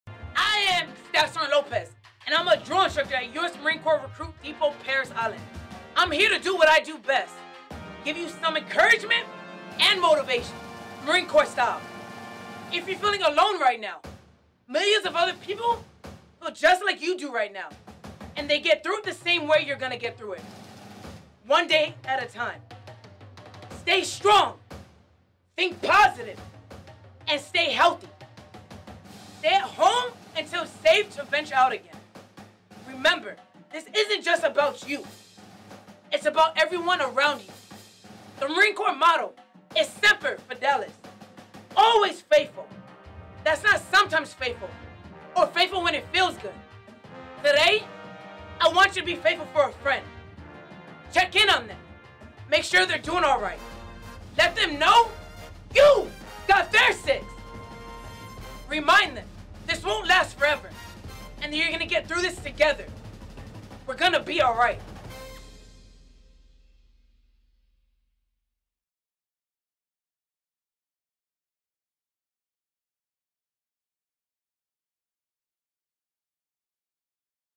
Isolation motivation from a 🇺🇸US Marine Corps drill instructor
It’s easy to lose motivation when you’re stuck at home. To keep you goal-oriented during these challenging times, US Marine Corps Staff Sergeant Lopez gives you some tough love, drill instructor style.

Made famous in popular culture, US Marine Corps drill instructors specialise in putting aspiring Marines through the rigours of boot camp. Lopez is responsible for training recruits at Marine Corps Recruit Depot Parris Island (South Carolina, USA), and she knows how to get people moving.